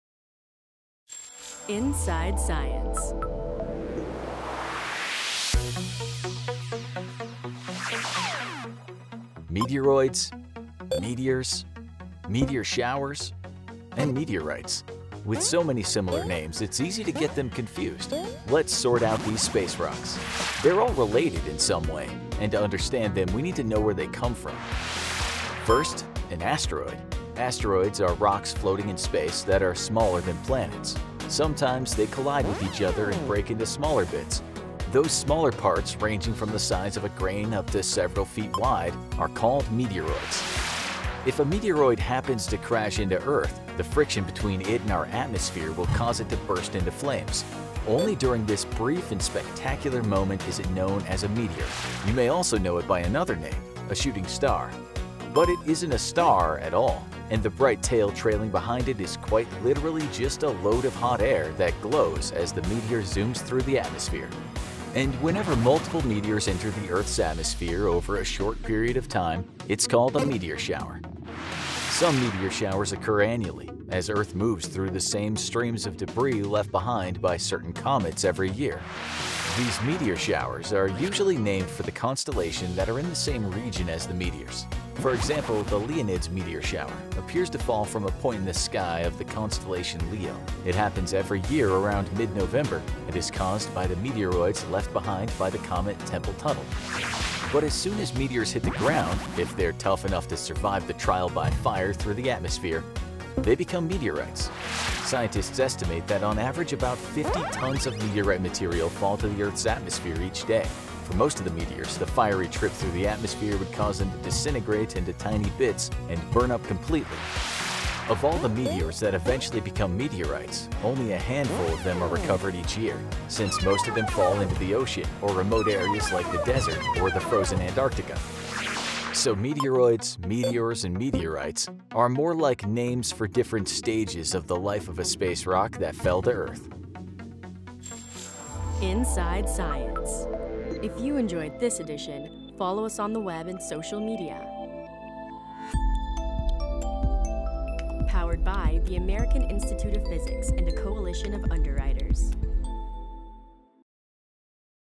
Meteoroids, Meteors, Meteor Showers and Meteorites
Sorting out space rocks and setting the record straight.

(Inside Science) -- Meteoroids … meteors … meteor showers … meteorites. With so many similar names, it's easy to get them confused. They're all related to the flashes of light called "shooting stars" sometimes seen streaking across the sky. But the same object we see has different names, depending on where it is. We sort out all the space rocks and help you make sense of it all.

Inside Science is an editorially independent news service of the American Institute of Physics